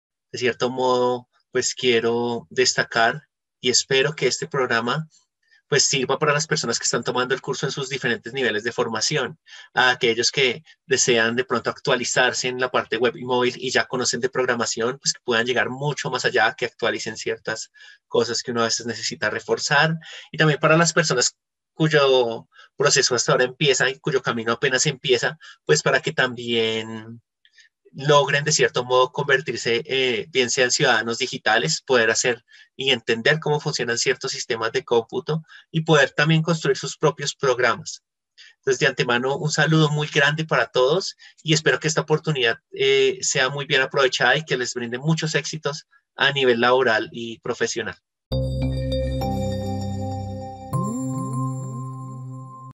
MisiónTIC, una oportunidad a nivel personal y laboral
Arles Ernesto Rodríguez, docente de la Universidad Nacional, comenta cómo MisiónTIC2022 tiene como objetivo actualizar y reforzar aprendizajes. También, para los nuevos integrantes sirve para entender el mundo digital y es una oportunidad muy buena a nivel personal y laboral.